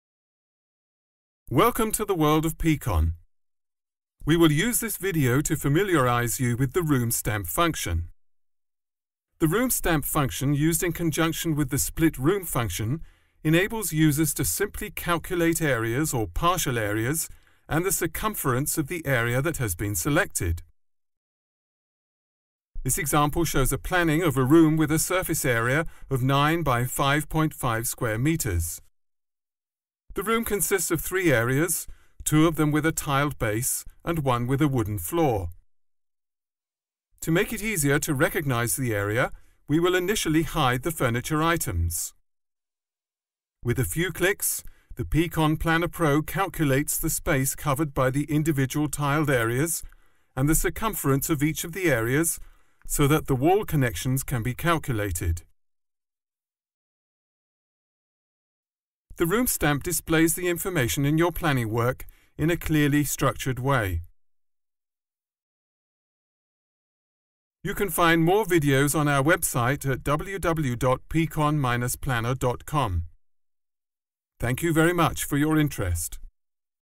Placing room stamps
We will use this video to familiarise you with the "room stamp" function of the pCon.planner PRO. The function "room stamp" used in conjunction with the function "split room" enables users to simply calculate areas or partial areas and the circumference of the area that has been selected.